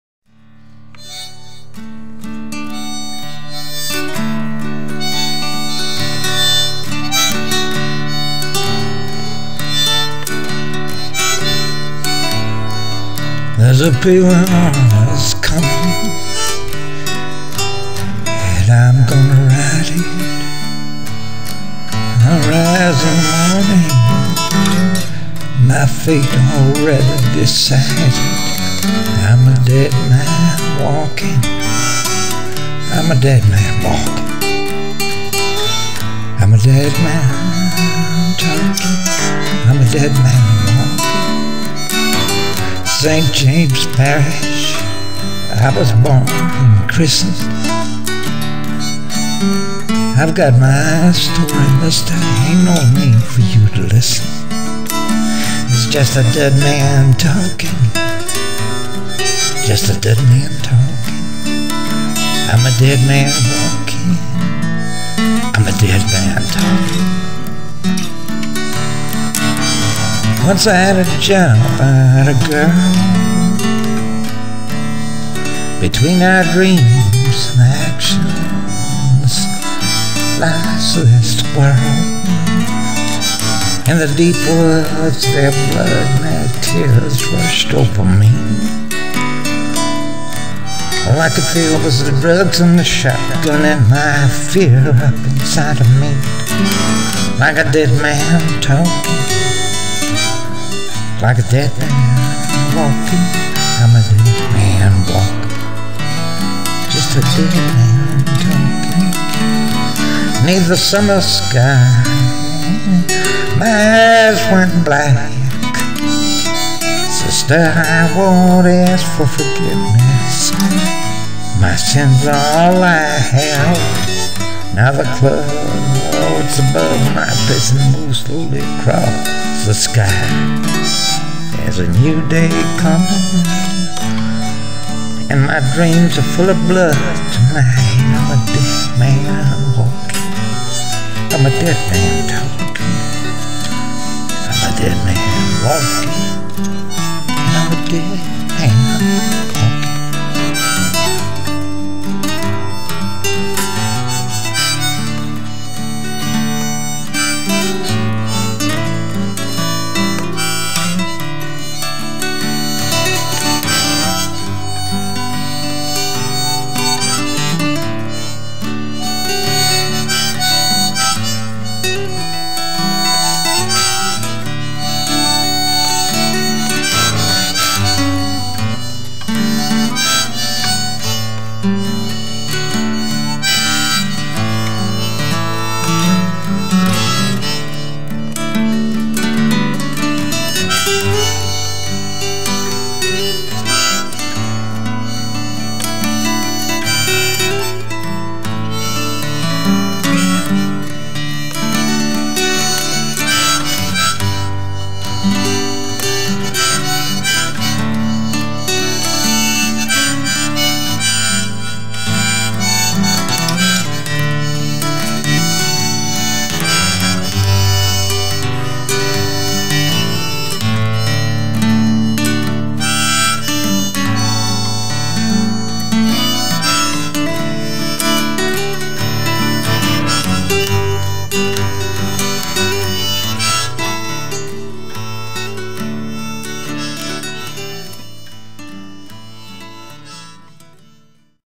"Dead Man Walkin'" 5 28 15
Elmo Patrick Sonnier was executed for the murder of two teenagers, a boy from St. Martinville, my home town, and his girlfriend, from New Iberia.  The boy was was my brother's next door neighbor.  I recall the case well.  Sister Helen Prejean was his spiritual adviser while he awaited his fate on Louisiana's death row.  His brother got life in prison because he was a minor at the time of the killings.  They'd pulled this before, pretending to be police officers and stopping young couples.  This was the first time rape turned to murder however, and other victims who'd remained silent came forward.